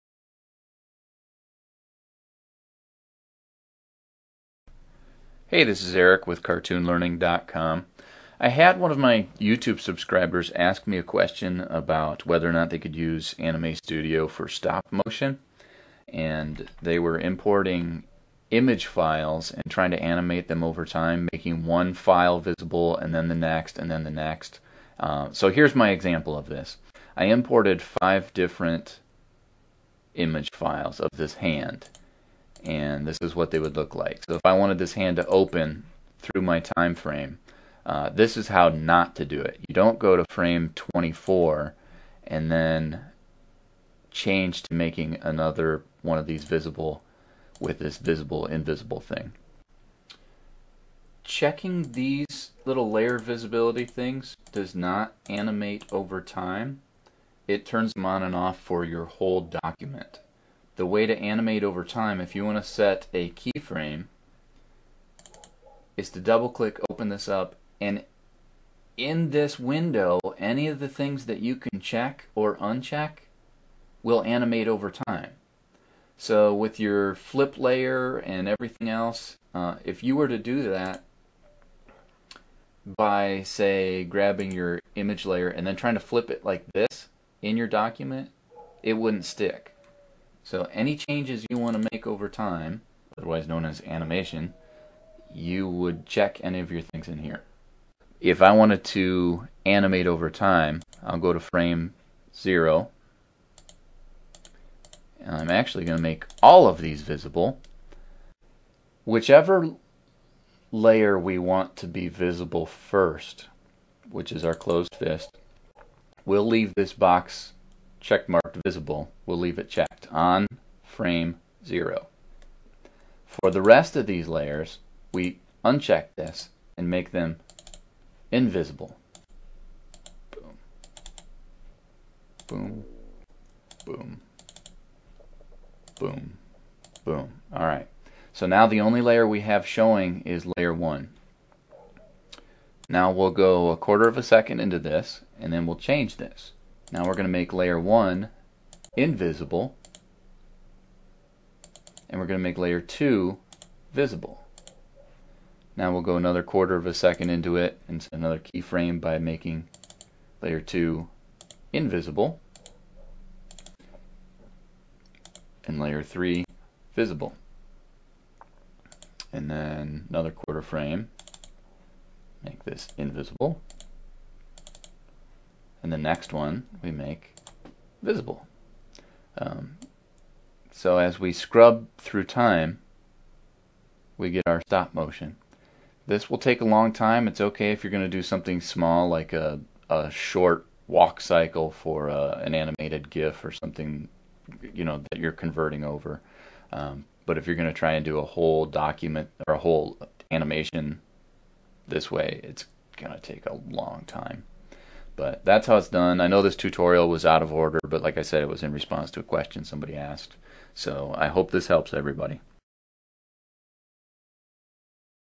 Anime Studio Tutorials : stop motion animation
Anime Studio tutorials on how to make stop motion animation with Anime Studio 7, or other versions.